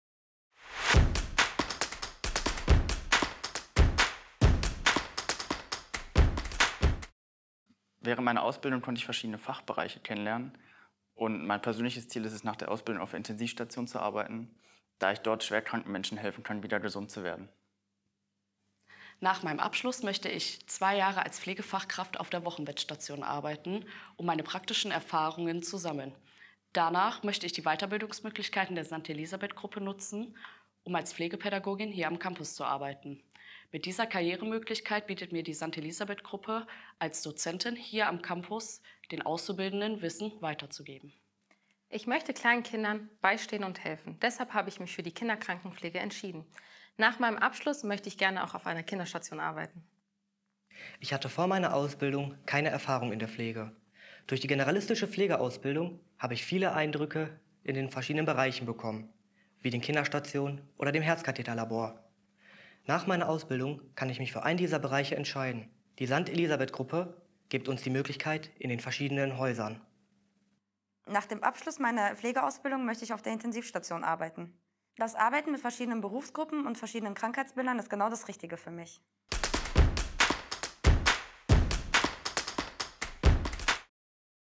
Pflegeausbildung – und dann? Pflegeauszubildende berichten von ihren Plänen nach der Ausbildung
Im heutigen Video stellen wir fünf Auszubildende vor, die derzeit eine Pflegeausbildung in der St. Elisabeth Gruppe absolvieren und kurz vor ihrem Examen stehen. In den Videos haben sie uns verraten, welche Pläne sie für die Zeit nach ihrem Abschluss haben.

So wollen sie beispielsweise auf Intensivstationen arbeiten, in der Gesundheits- und Kinderkrankenpflege tätig werden oder die vielfältigen Weiterbildungsmöglichkeiten des Unternehmens nutzen, um ein Studium der Pflegepädagogik zu beginnen.

Neugierig geworden? Bewerbungen sind über das Jobportal möglich